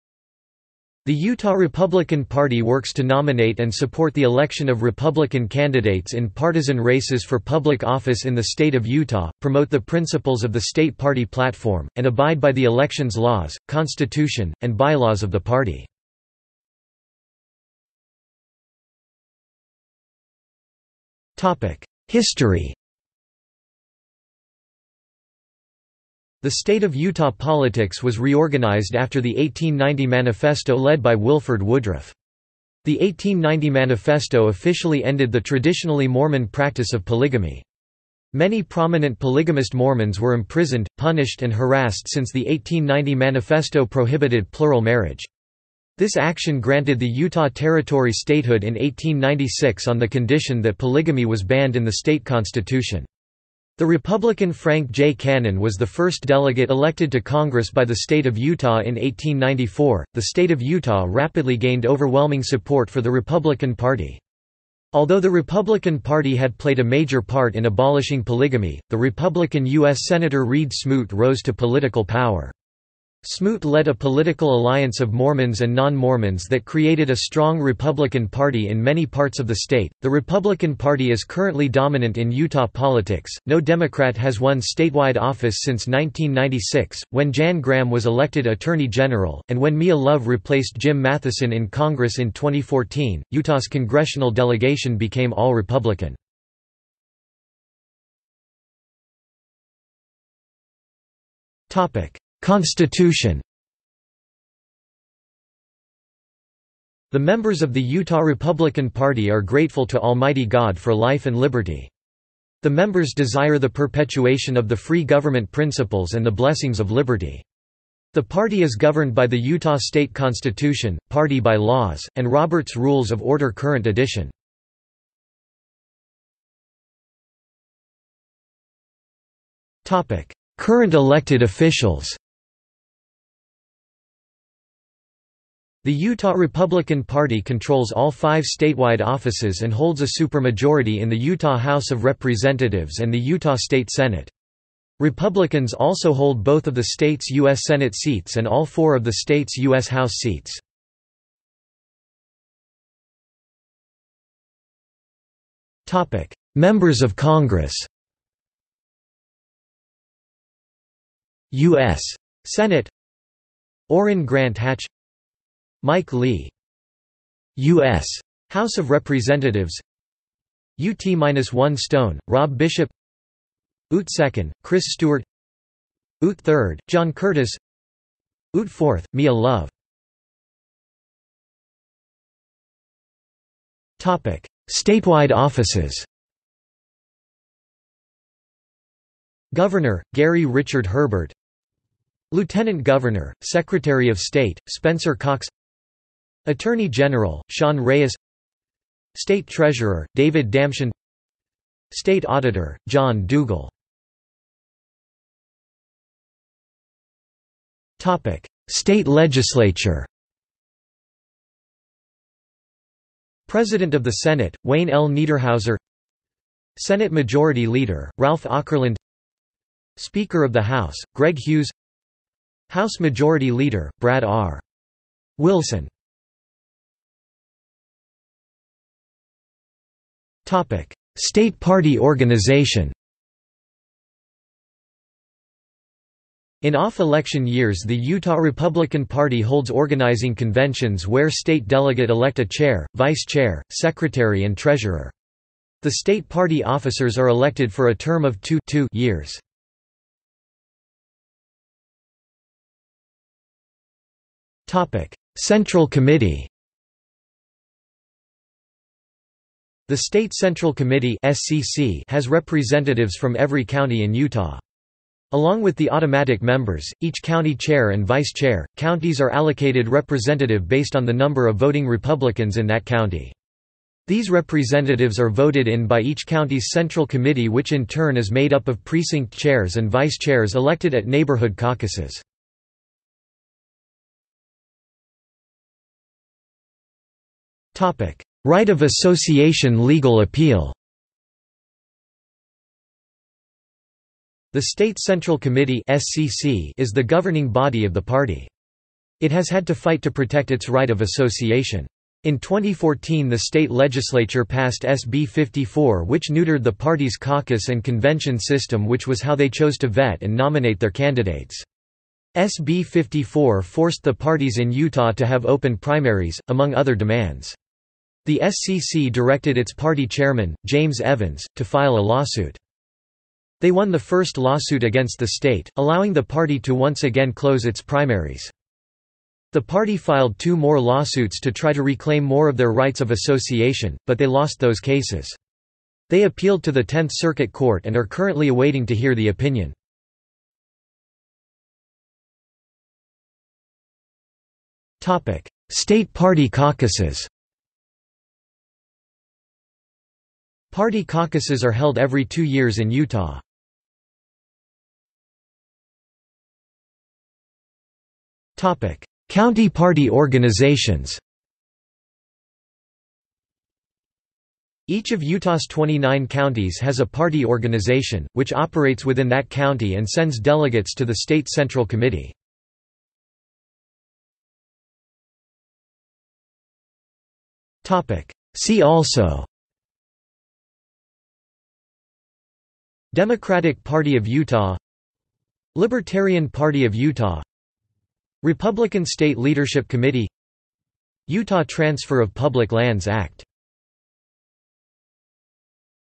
Utah Republican Party | Wikipedia audio article
This is an audio version of the Wikipedia Article:
Utah Republican Party

00:00:17 1 History
00:01:38 2 Constitution
00:02:06 3 Current elected officials
00:02:32 3.1 Members of Congress
00:02:41 3.1.1 U.S. Senate
00:02:47 3.1.2 U.S. House of Representatives
00:03:00 3.2 Statewide offices
00:03:24 3.3 State Legislature
00:03:46 4 State party organization
00:04:11 4.1 Central Committee
00:04:44 5 Right of Association Legal Appeal
00:05:41 5.1 State Party Caucuses
00:05:54 6 County party organizations
00:06:14 7 See also

- Socrates

SUMMARY
The Utah Republican Party works to nominate and support the election of Republican candidates in partisan races for public office in the state of Utah; promote the principles of the State Party Platform; and abide by the elections laws, constitution, and bylaws of the Party.